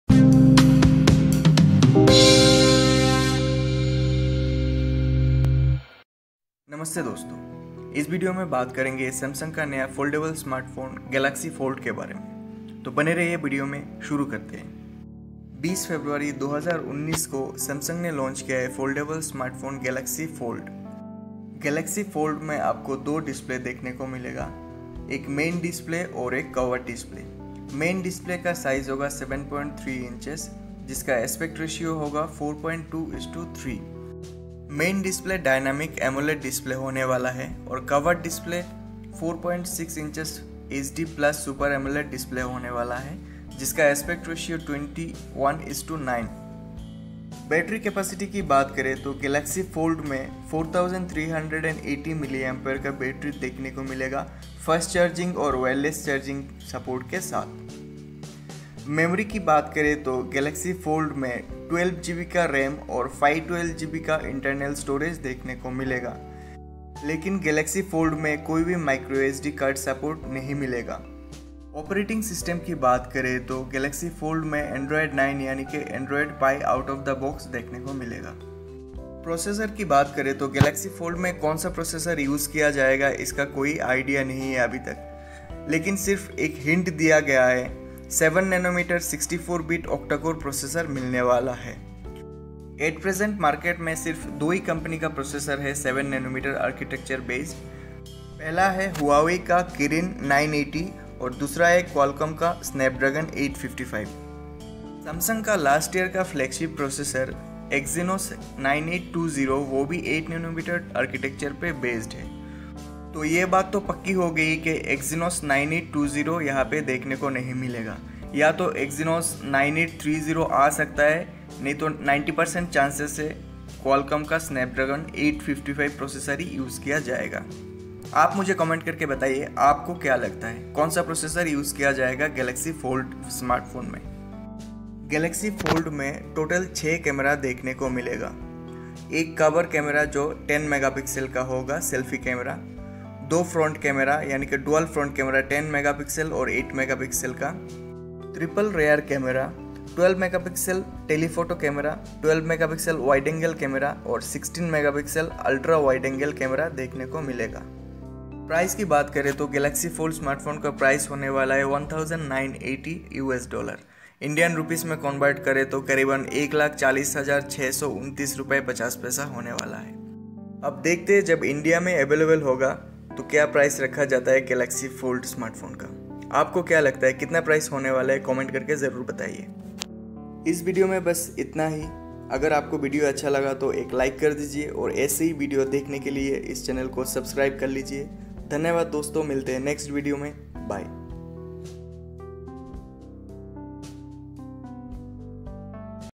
Samsung Galaxy Fold 2019 Foldable Smart Phone First look Detail Video with Specifications in Hindi
Samsung Galaxy Fold 2019 Foldable SmartPhone First look Detail Video with Specifications in Hindi

NOTE:
NOTE:
If You have any problem or Issue with this video, then KINDLY let me know before taking any action.